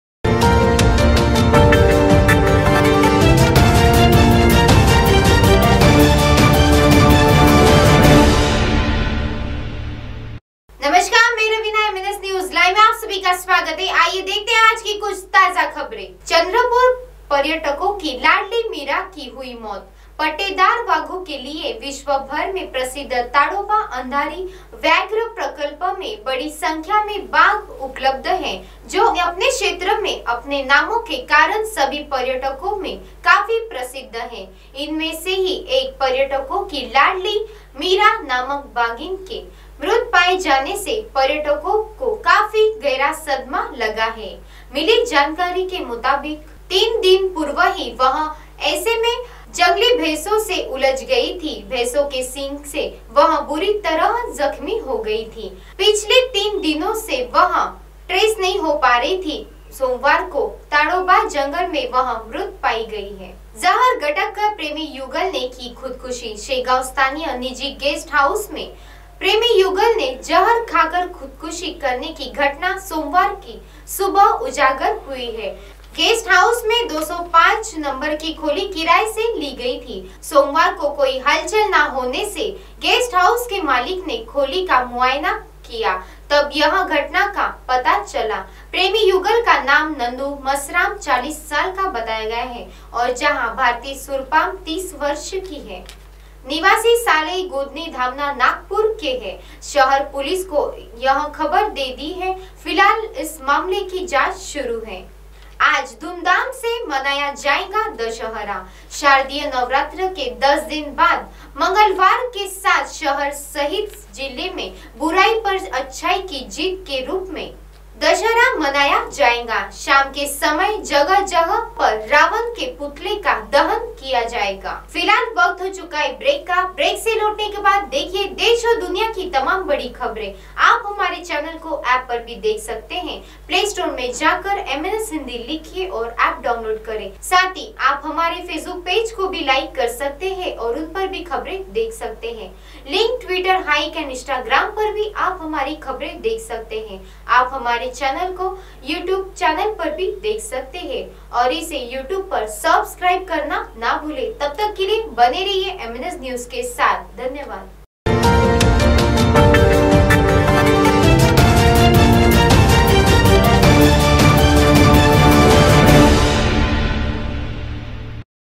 पर्यटको की लाड़ली मीरा की हुई मौत।
MNSNEWSLIVE चंद्रपुर पर्यटको की लाड़ली मीरा की हुई मौत।